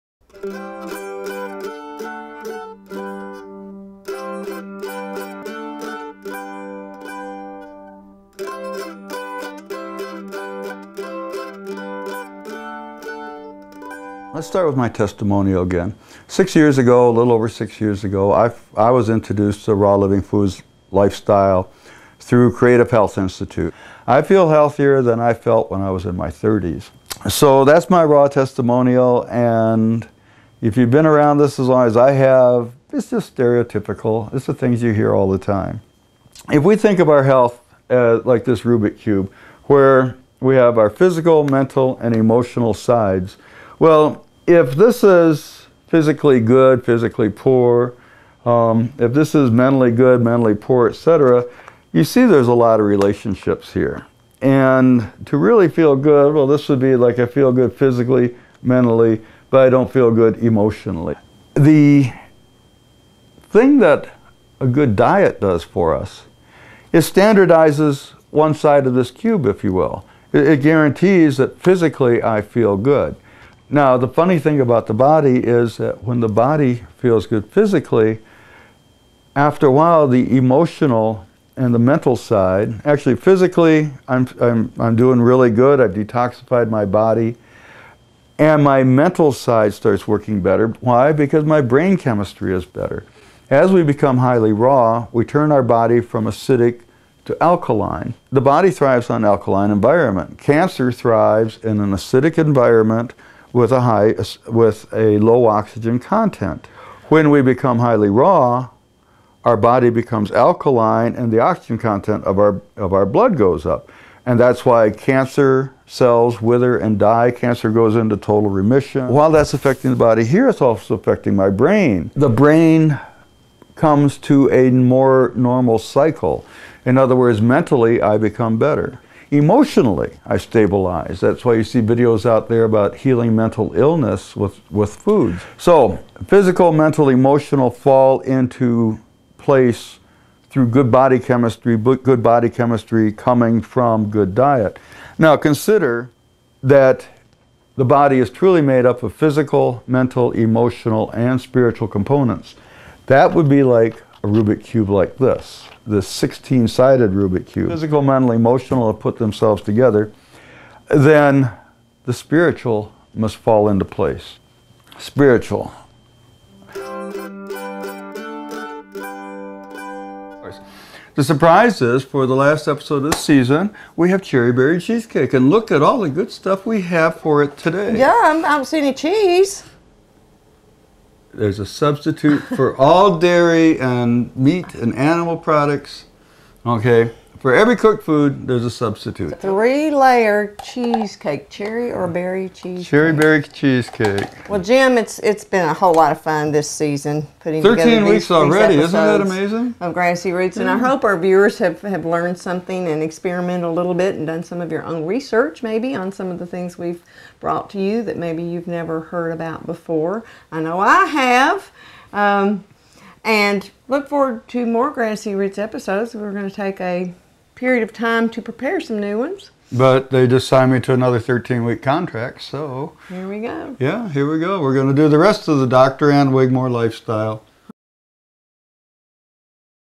Putting It All Together
Dr. Jim ties it all together - what you eat, how you eat, where you eat, your outlook on life, and even the spiritual aspects of the Living Foods Lifestyle. He shows how all of this is related to good, optimal health and healing, and has some suggestions for you. Excerpts from GrassyRootsTV.com.